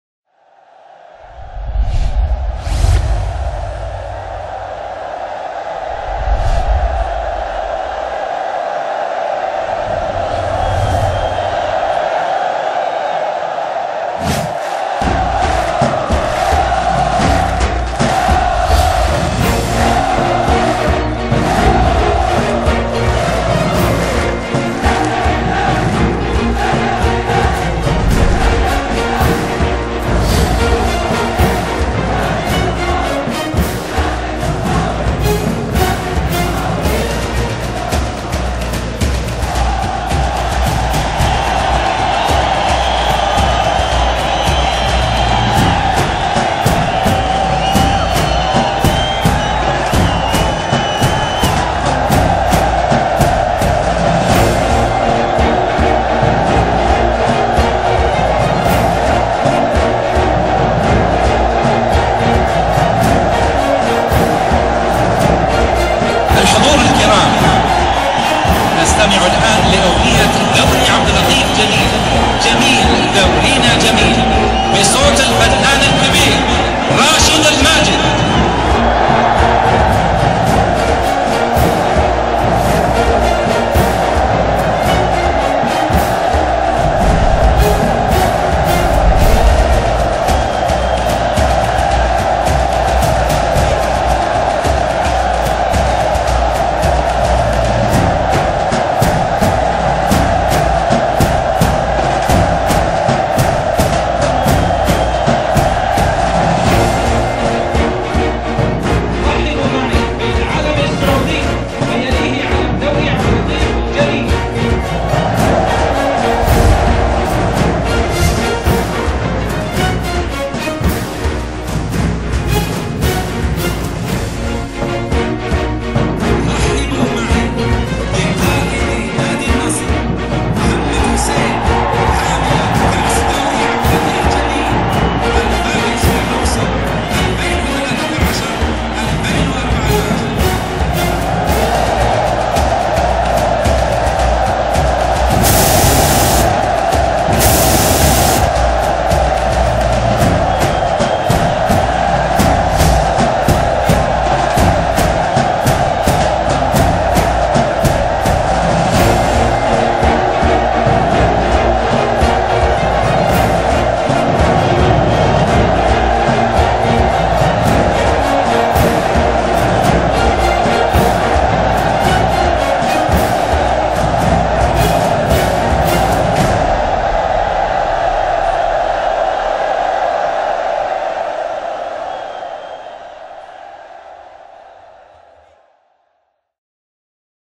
ALJ League Opening Ceremony 2014 15
An amazing Opening Ceremony for the ALJ league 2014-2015